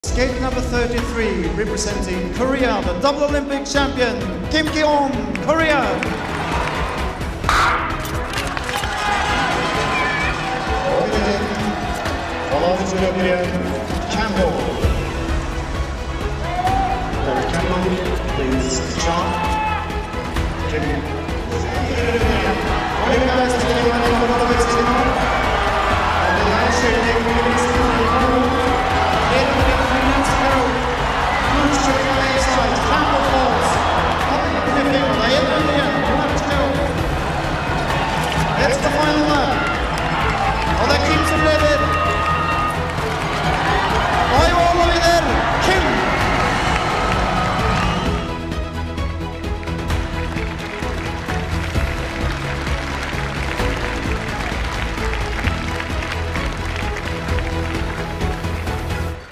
N년 전 오늘⏰ 쇼트트랙 1000m 2연패 달성의 순간! 1994릴레함메르 쇼트트랙 1000m
1994 릴레함메르, 쇼트트랙 1000M 김기훈⛸